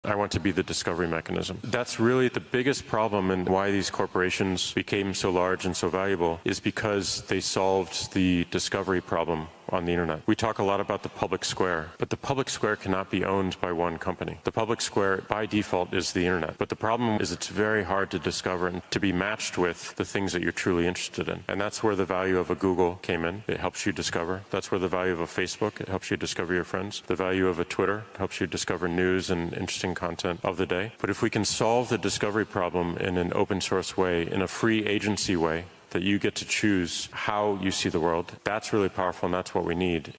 Own your discovery mechanism
Having the ability to choose our discovery mechanism seems like the solution

We just need to ensure all the content is accessible, which is the leverage existing corporations have